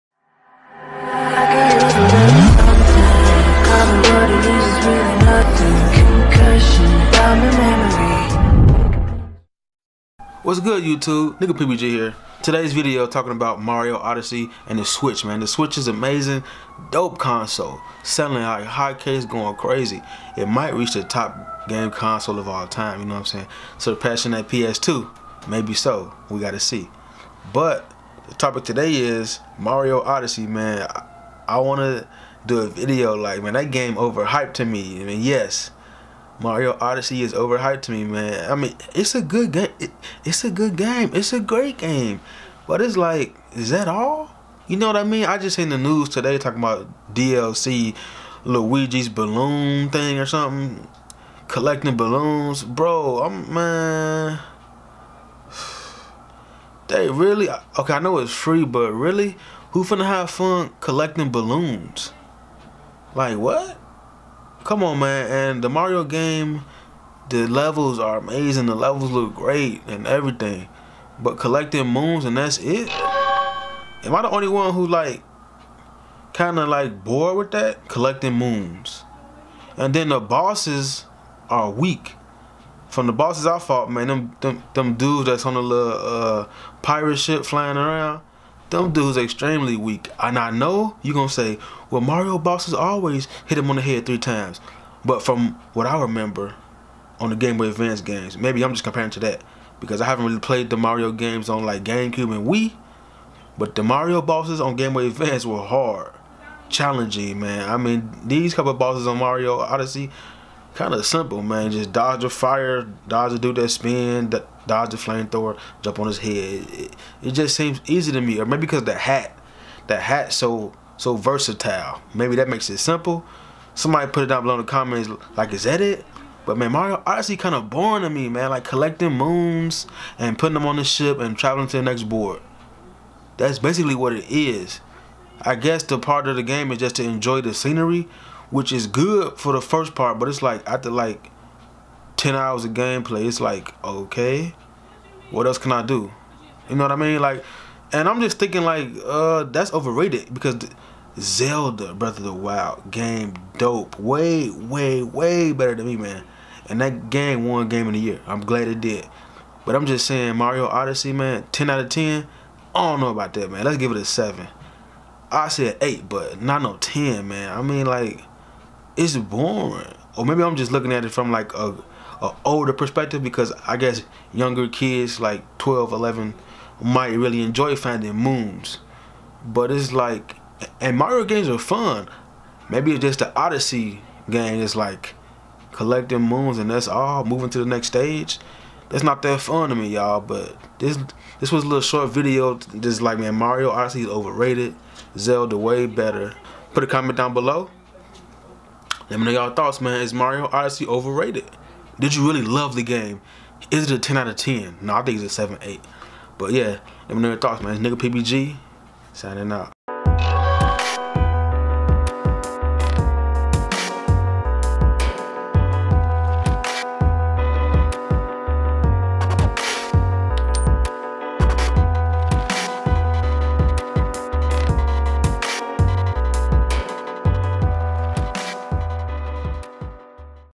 Mario Odyssey is Overrated and Overhyped! Thoughts?
Mario Odyssey is Overrated and Overhyped! Thoughts? The so called best nintendo game!! What about Zelda BOTW. Mario odyssey on Nintendo Switch is Overrated and overhyped!!! i will explain!!! is it really a 10/10 game?